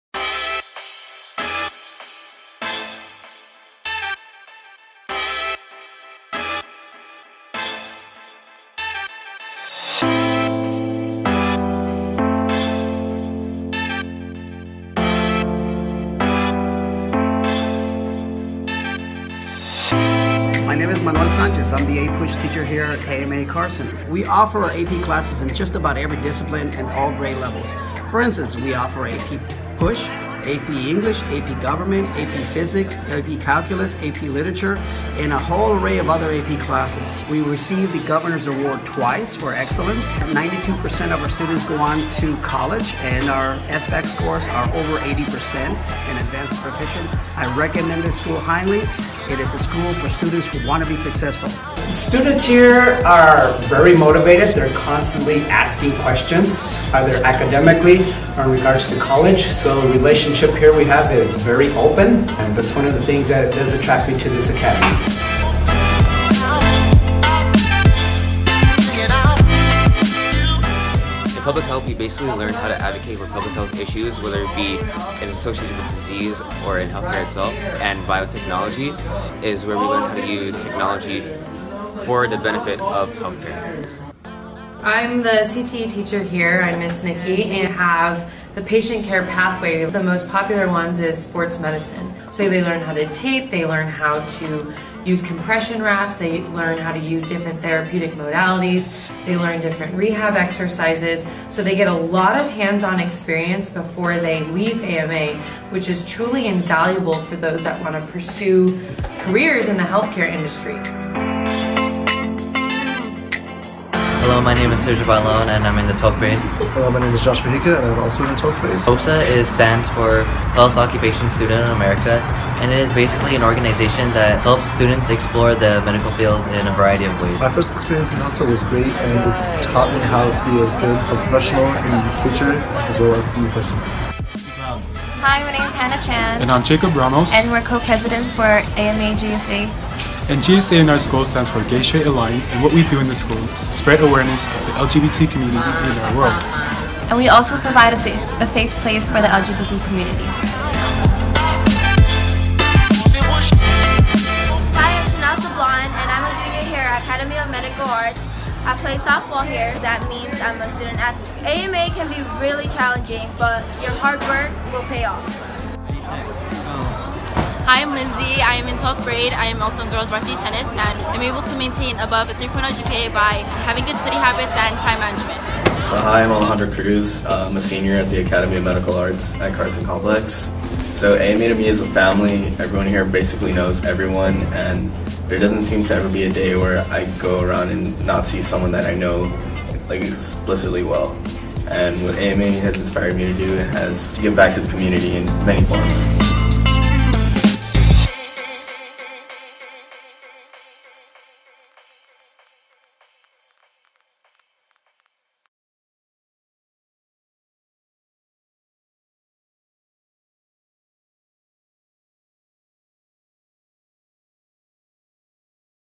Zone of Choice Academy of Medical Arts
Academy of Medical Art's Zone of Choice Video. Created entirely by a team of students that were given the task of creating this video.

Directors are:
Manuel A.
Justin Battle.
Assistants:
Matthew Y.
Fiorina T.

Thanks to the teachers and students who participated and coordinated the project. And like always, ManWhale signing off.